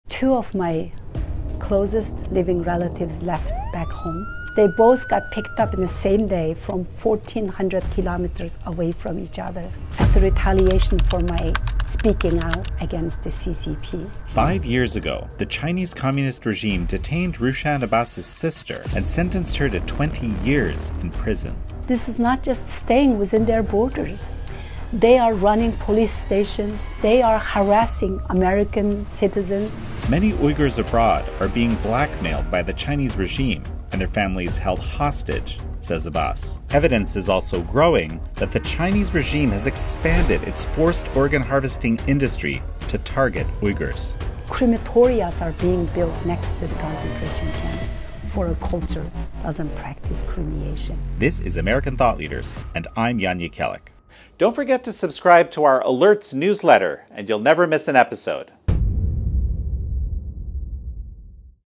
My Sister’s Abduction: Rushan Abbas on the CCP’s Tactics to Control the Uyghur Diaspora | TEASER
Back in September 2018, just days after Rushan Abbas spoke at a Hudson Institute panel about the genocide of the Uyghurs, authorities in China arrested her sister and her aunt.

“Two of my closest living relatives left back home, they both got picked up on the same day from 1,400 kilometers away from each other … as retaliation for my speaking out,” Ms. Abbas says.

Her sister was sentenced to 20 years in prison.

Many Uyghurs abroad are being blackmailed by the Chinese regime and their families held hostage, Ms. Abbas says. She is the founder and executive director of Campaign for Uyghurs.

Evidence is also mounting that the CCP is killing large numbers of Uyghurs for their organs.

How My Sister Was Abducted: Rushan Abbas on the CCP’s Tactics to Control and Manipulate the Uyghur Diaspora

🔴 $1 for 2 months - Try EpochTV Today!